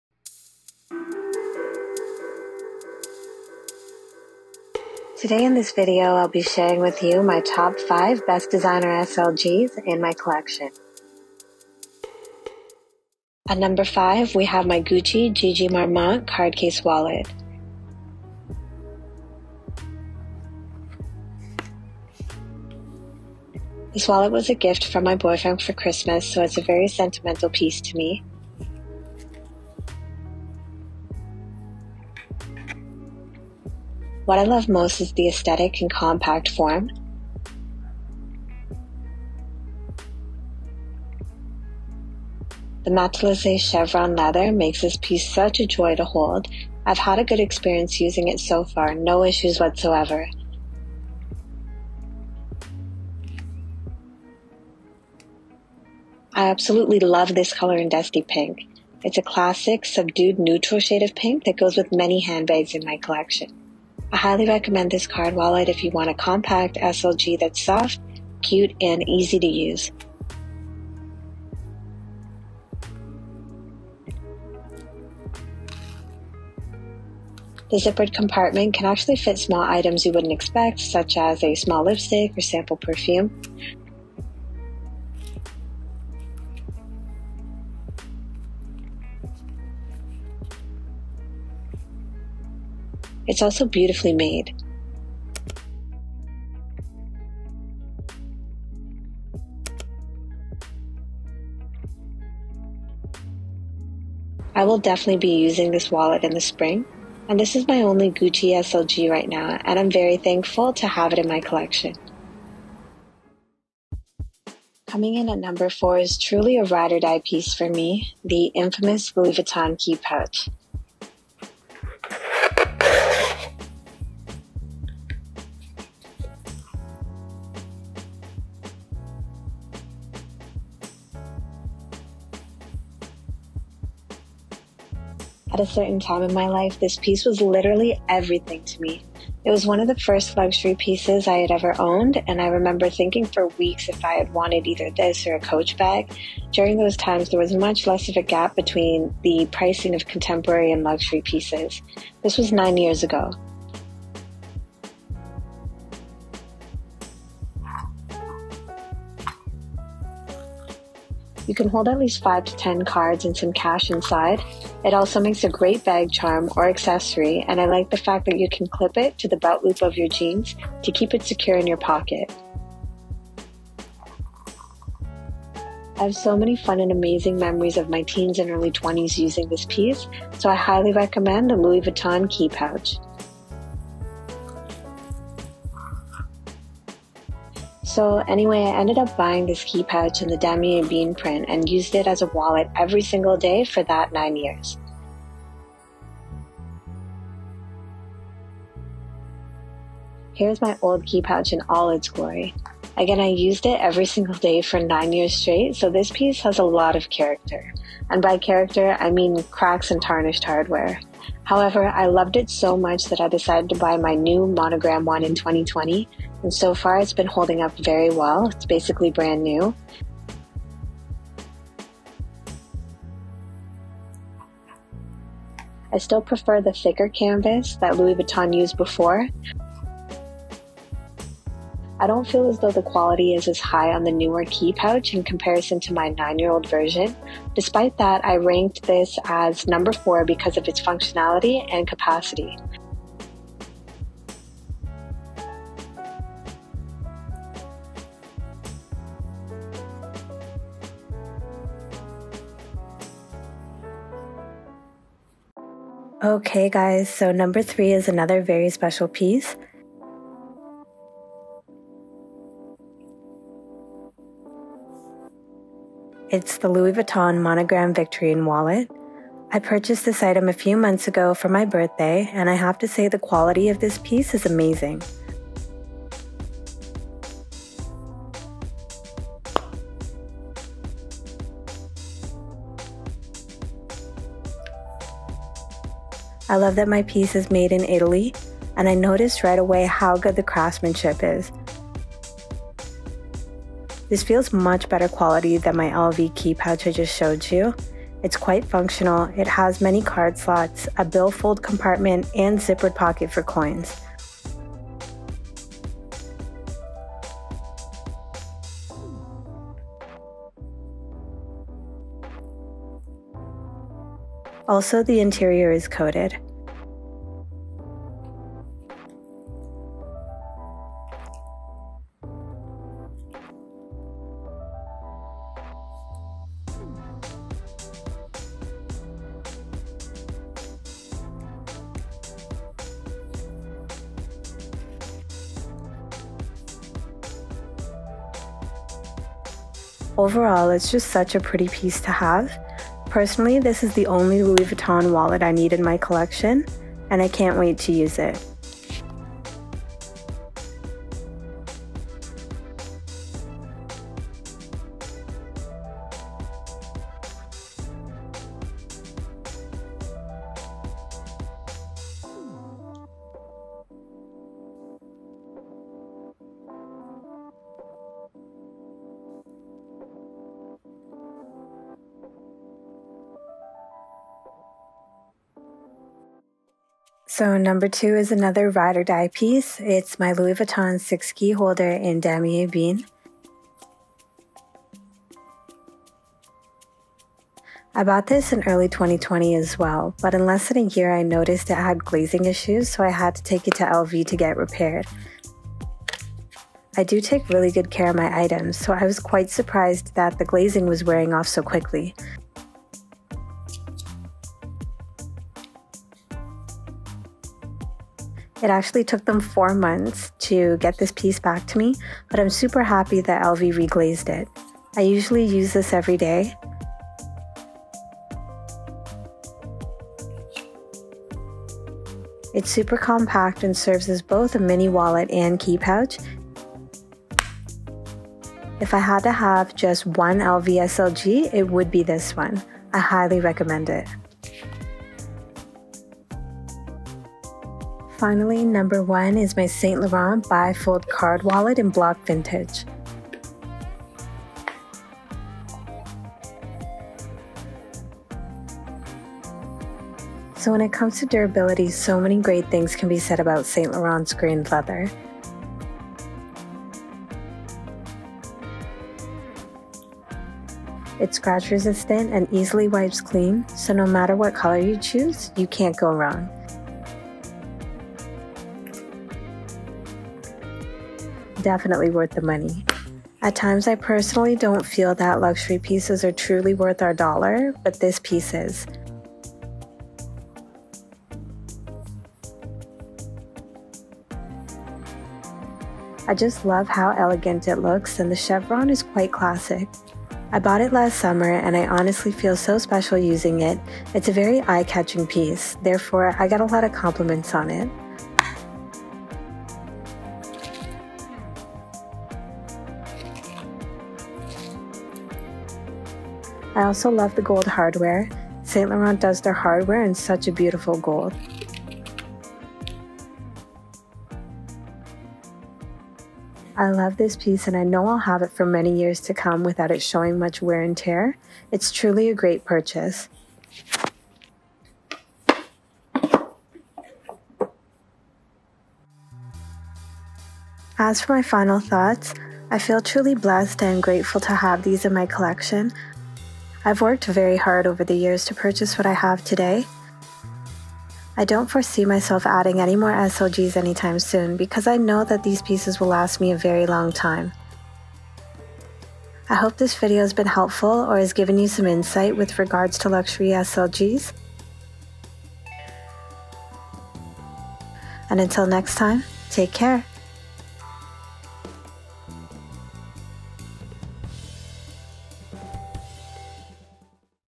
MY 5 BEST DESIGNER SLGS ♡ louis vuitton, gucci, saint laurent *forever pieces*
Here are my top 5 best luxury small leather goods in my collection ranked. From Louis Vuitton, Gucci, and Saint Laurent. These small leather goods were purchased directly from Louis Vuitton and Saint Laurent. My boyfriend bought me the Gucci Marmont Card Wallet on MatchesFashion. Links to their respective websites are down below. 🍯

5. Gucci GG Marmont Card Case Wallet - Dusty Pink

4. Louis Vuitton Key Pouch - Monogram M62650

3. Louis Vuitton Victorine Wallet - Monogram M62472

2. Louis Vuitton 6 Key Holder - Damier Ebene N62630

1. Saint Laurent Monogram Bi-Fold Wallet - Grain De Poudre